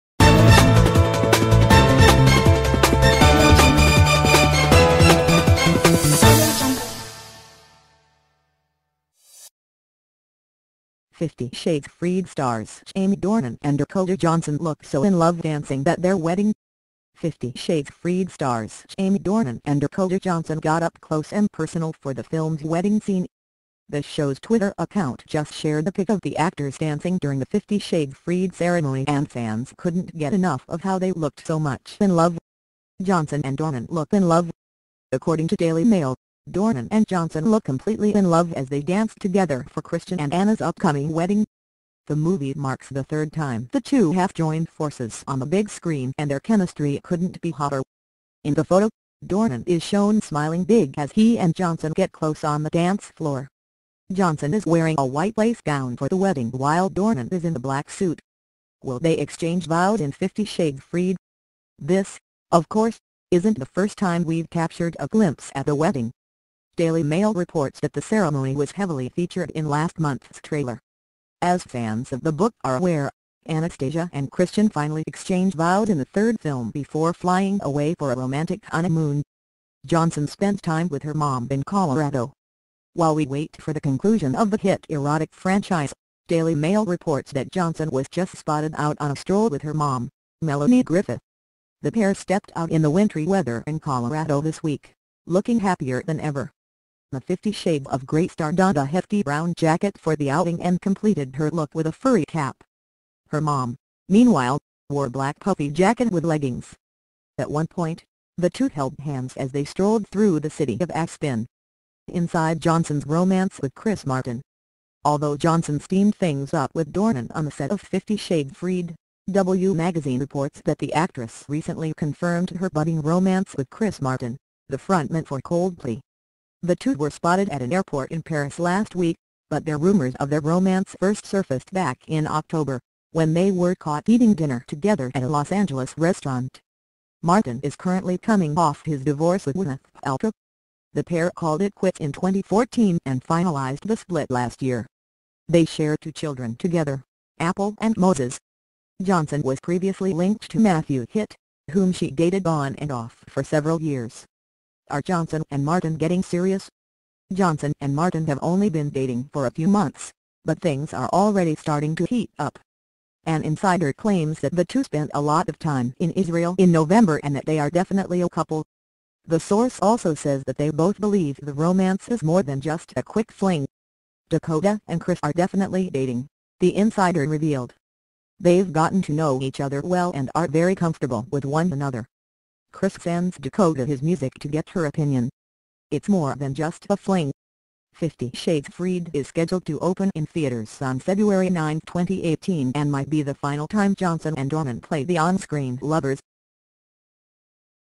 What did Chris Martin think of Jamie Dornan and Dakota Johnson in the wedding scene?
Fifty Shades Freed stars Jamie Dornan and Dakota Johnson got up close and personal for the film’s wedding scene